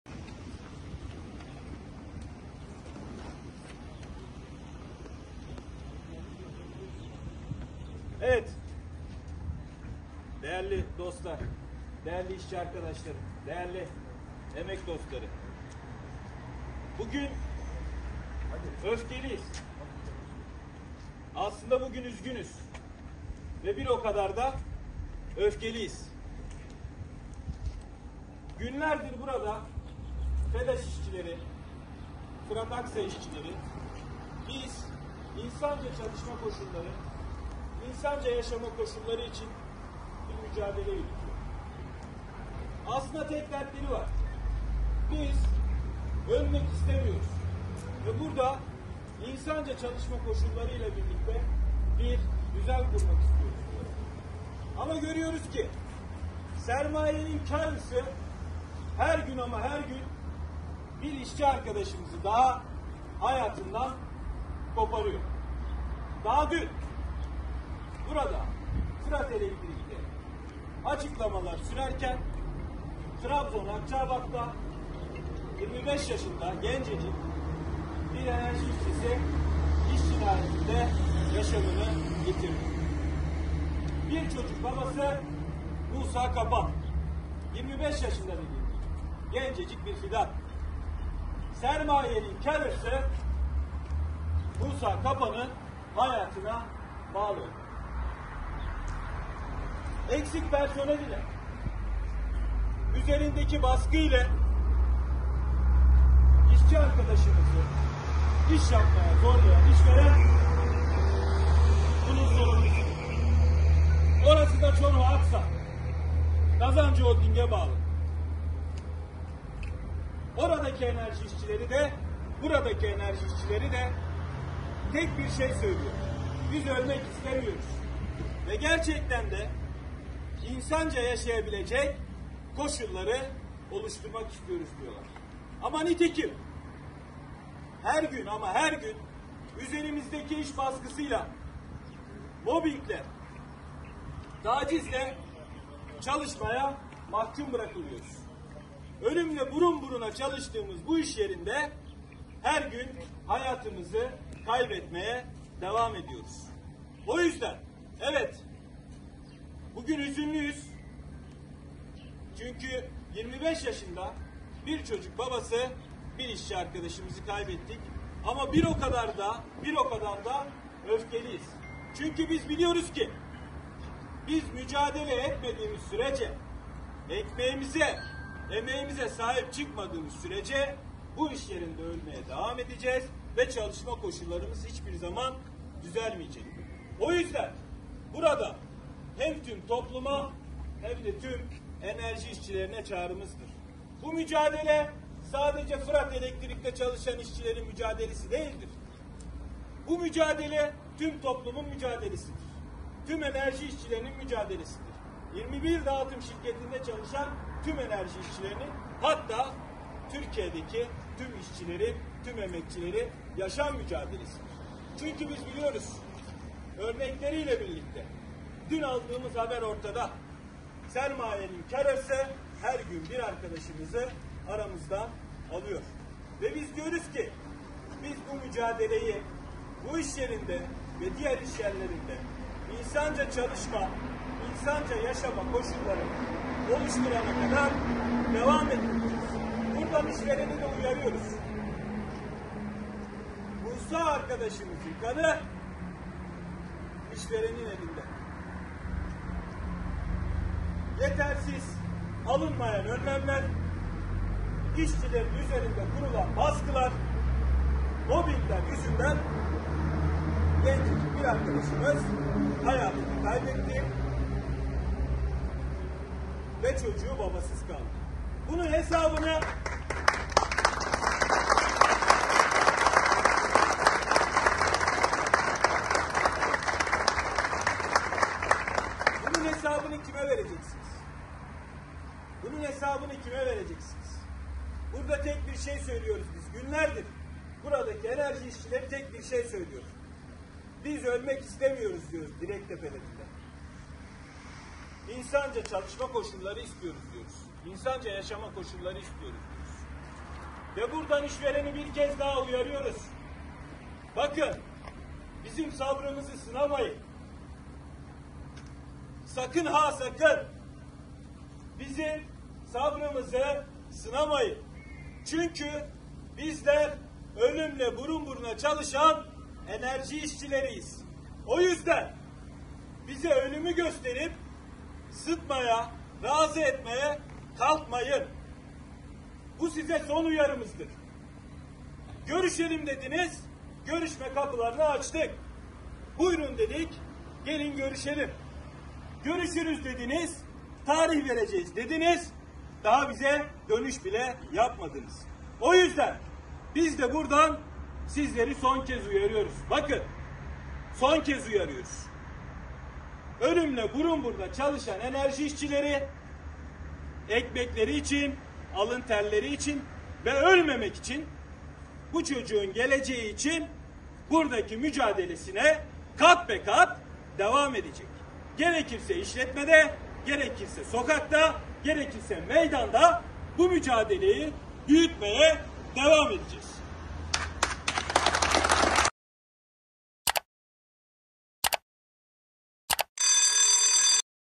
Tunceli’de grevde olan FEDAŞ İşçileri Trabzon'da ölen Musa Kasap için Basın Açıklaması yaptı
Trabzon’da Çoruh EDAŞ işçisi elektrik direkleri arasına kablo çekmek isterken tırmandığı direkte elektrik akımına kapılarak hayatını kaybetti.

Bizi;
Twitter'da: TunceliEMEK

Facebook'ta:
Tunceli EMEK Gazetesi

Lınkedın'da
tunceliemekgazetesi adlı sayfalarımızdan takip edebilirsiniz